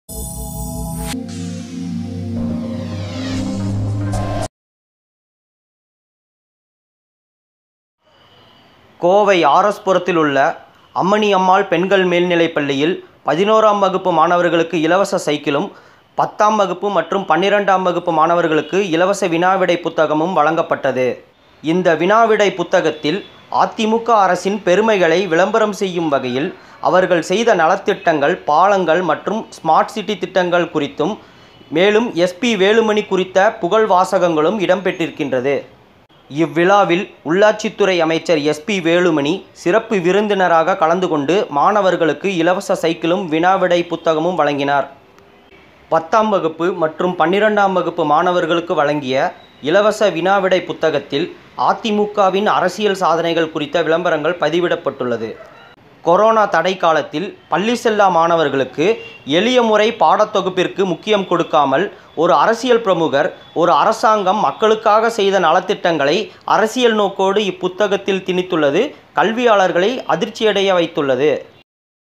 10,+2 புக்கில் ADMK விளம்பரம்|அரசியல் புகழ் பாடும் கல்வி புத்தகம்
10ஆம் வகுப்பு மற்றும் 12ஆம் வகுப்பு இலவச வினாவிடை கெய்டில், அதிமுக வின் அரசியல் சாதனைகள் குறித்த விளம்பரங்கள் பதிவிடப்பட்டுள்ளது.

கோவை ஆர்எஸ் புரத்தில் உள்ள அம்மணி அம்மாள் பெண்கள் மேல்நிலைப் பள்ளியில் பதினொண்னாம் வகுப்பு மாணவர்களுக்கு இலவச சைக்கிள்கள் வழங்கும் நிகழ்ச்சி நடைபெற்றது.10ம் வகுப்பு,மற்றும் 12ஆம் வகுப்பு மாணவர்களுக்கு இலவச வினாவிடை  கைடும் வழங்கப்பட்டது.

 இவ்விழாவில் உள்ளாட்சி துறை அமைச்சர் எஸ் பி வேலுமணி சிறப்பு விருந்தினராக கலந்து கொண்டு மாணவர்களுக்கு வழங்கினார்.

இந்த  கைய்டில்,அதிமுக அரசின் பெருமைகளைப் விளம்பரம் செய்யும் வகையில் அவர்கள் செய்த நல திட்டங்கள்,பாலங்கள் மற்றும் ஸ்மார்ட் சிட்டி திட்டங்கள், மேலும் அமைச்சர் எஸ் பி வேலுமணி குறித்த புகழ் வாசகங்களும் இடம் பெற்றிருக்கின்றது.
கொரோனா தடைகாலத்தில் பள்ளி செல்லா மாணவர்களுக்கு,எளிய முறையில் பாட தொகுபிற்க்கு முக்கியத்துவம் கொடுக்காமல், ஒரு அரசியல் பிரமுகர், ஒரு அரசாங்கம் மக்களுக்காக செய்த நலத்திட்டங்களை அரசியல் நோக்கோடு இப்புத்தகத்தில் திணித்துள்ளது கல்வியாளர்களை அதிர்ச்சி அடைய வைத்துள்ளது.